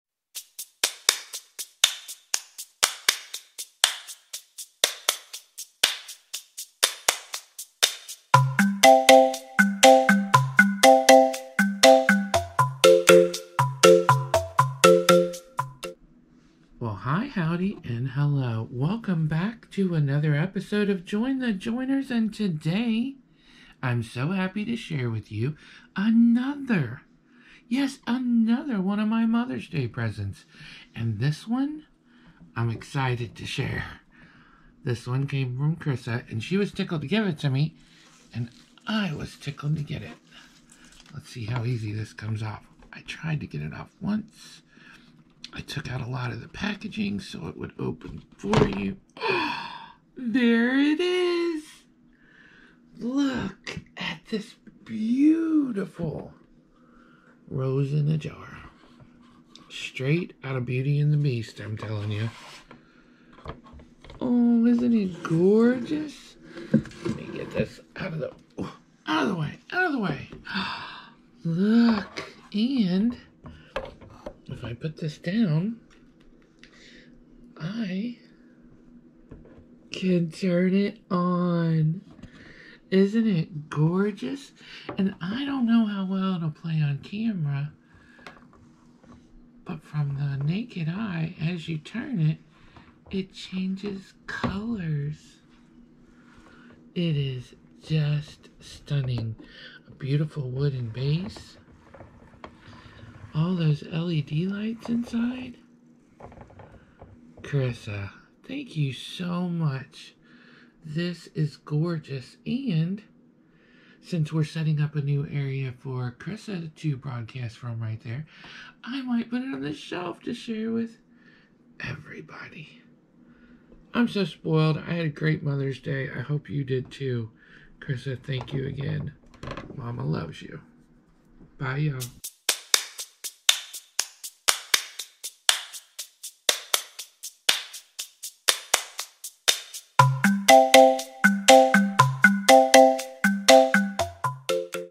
What did she get? OMG! What a beautiful gift!
This was one of my amazing presents from Chrissa this year for Mother's Day.